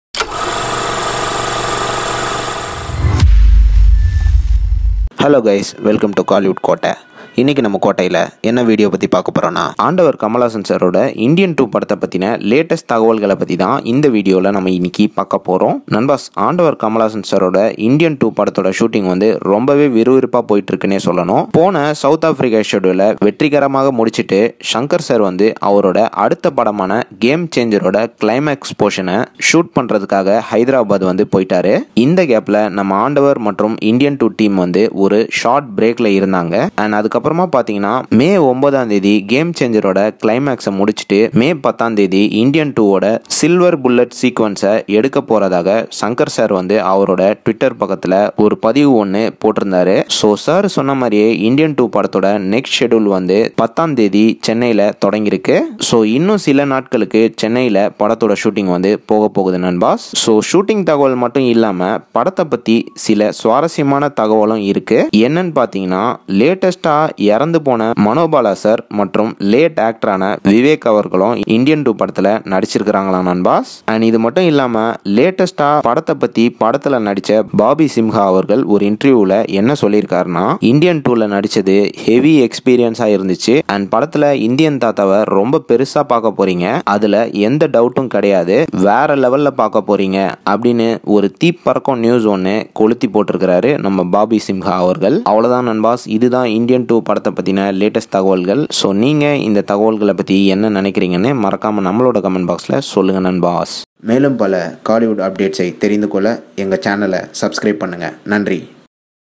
🤞Indian2 - Massana Latest Fire Update | Kamal Indian2 Movie Latest Update | Kamal Haasan
🤞Indian2 -  Massana Latest Fire Update | Kamal Indian2 Movie Latest Update | Kamal Haasan | Anirudh | Kollywood Kotta

Watch Kamal Haasan's 🤞Indian2 - Massana Latest Fire Update | Kamal Indian2 Movie Latest Update New Latest Update News Video On Our KollywoodKotta Channel. 🤞Indian2 is an upcoming 2022 Kollywood Tamil Film Directed by Shankar Ft. Kamal Haasan And Many More. The Film was Produced by Lyca Productions. The Music for the film is Composed by Anirudh Ravichander.